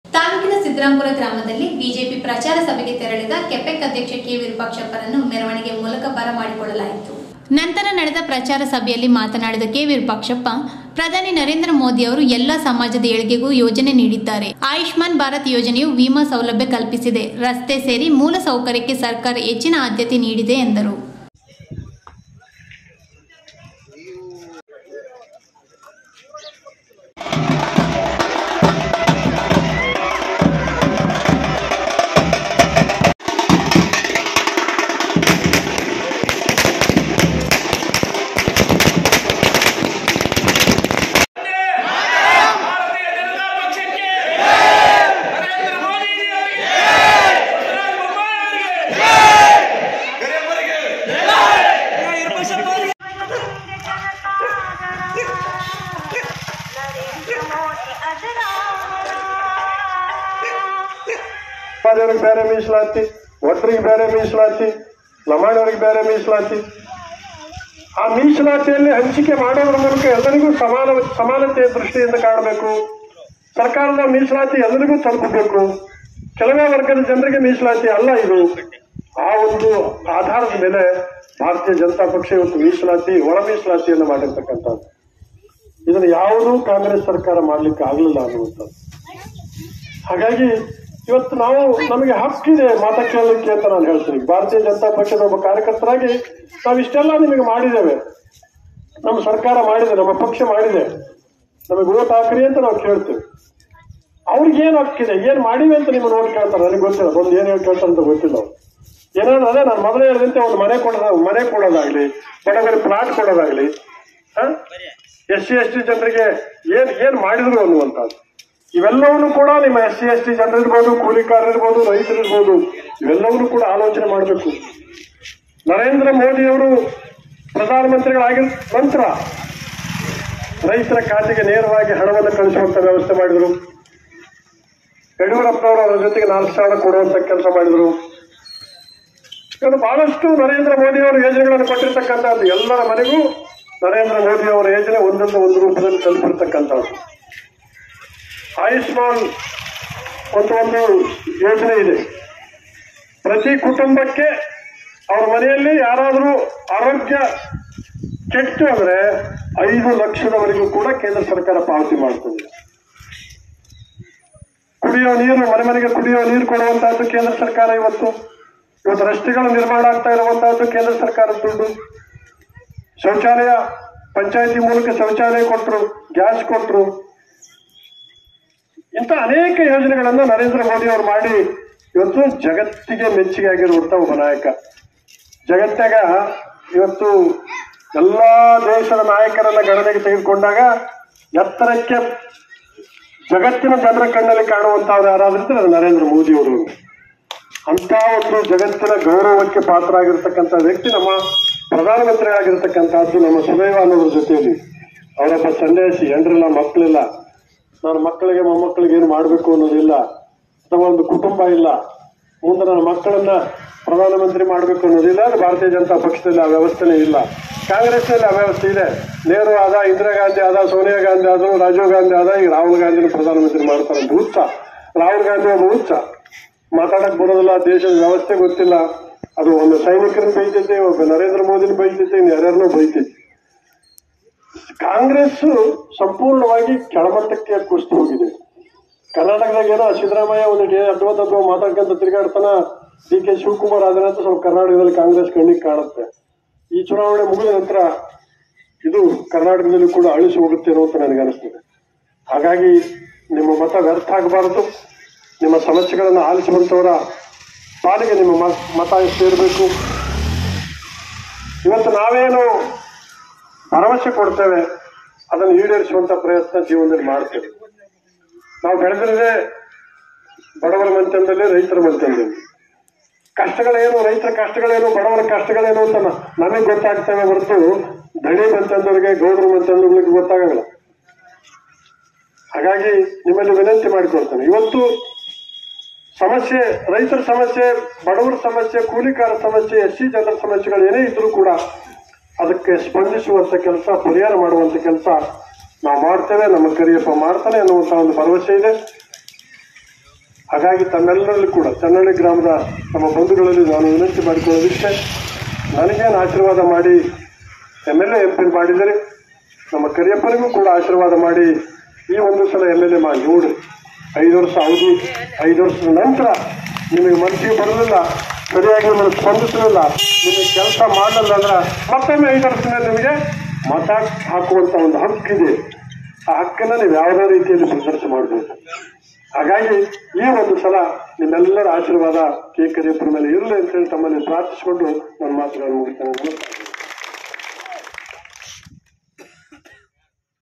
ಪ್ರಧಾನಿ ನರೇಂದ್ರ ಮೋದಿಯವರ ಆಡಳಿತ ಜಗತ್ತಿಗೆ ಮಾದರಿ: ಕೆಪೆಕ್ ಅಧ್ಯಕ್ಷ ಕೆ.ವಿರೂಪಾಕ್ಷಪ್ಪ
Reporter Ambanna#sasalamari